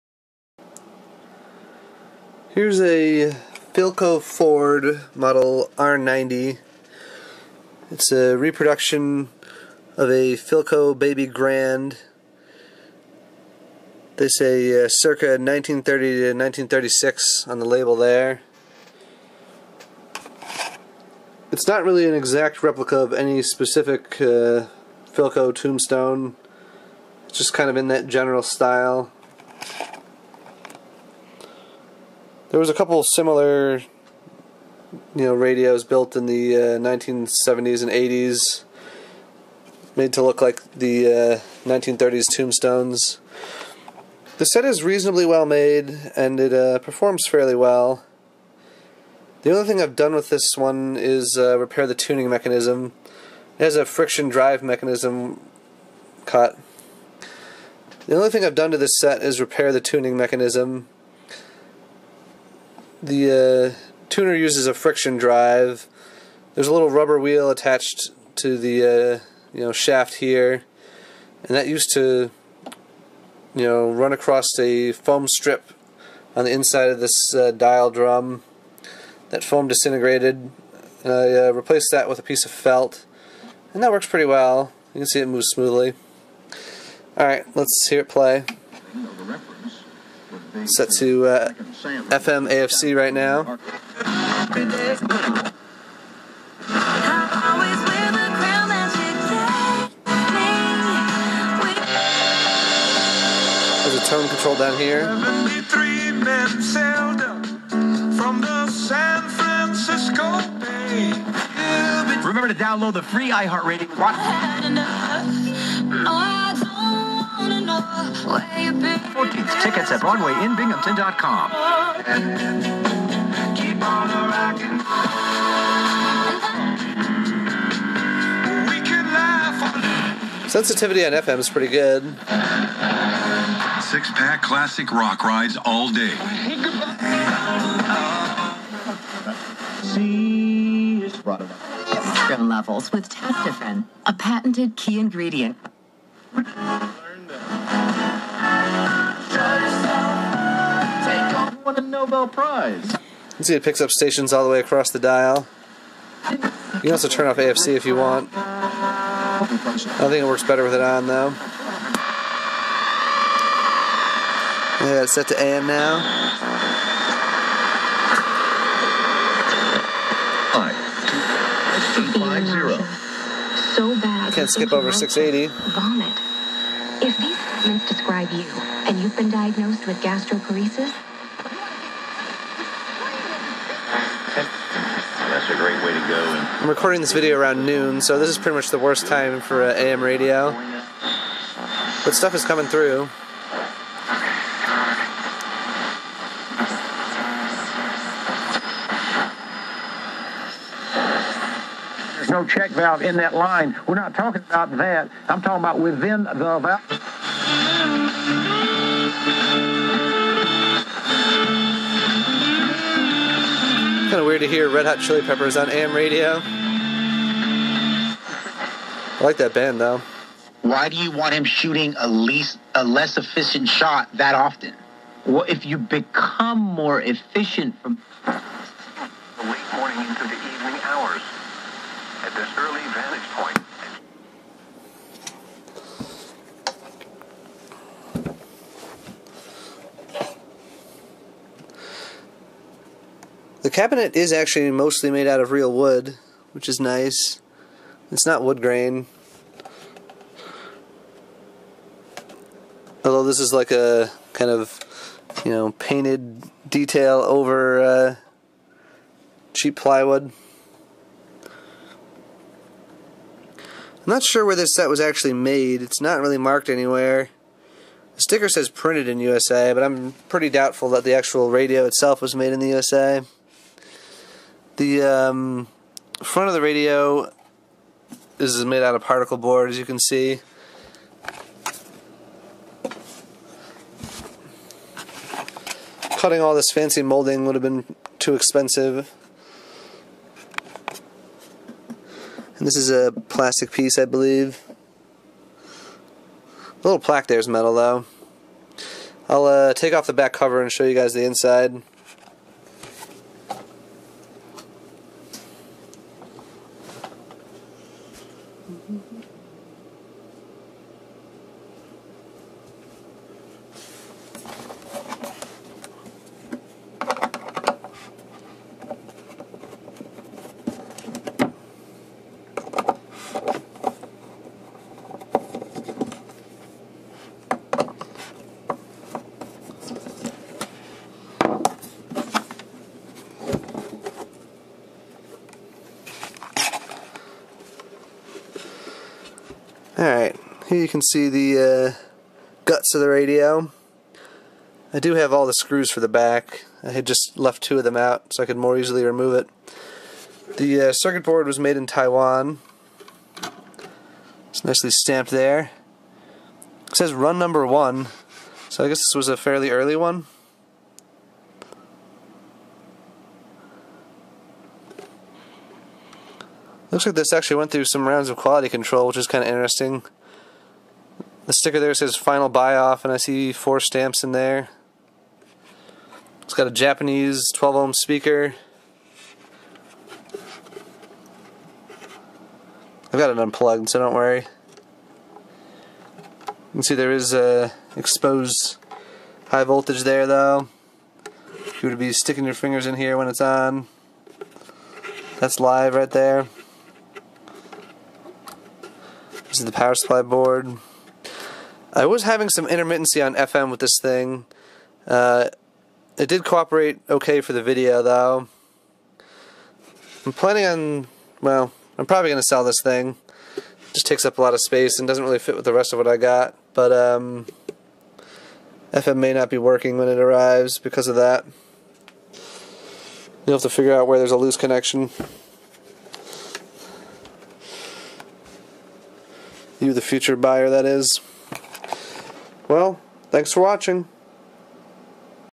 Philco-Ford model R-90 cathedral transistor radio (~1973, Taiwan)
This radio was sold in the early to mid 70's, and is all transistor (no integrated circuits or tubes).
Sorry for the shaky camera in the second half.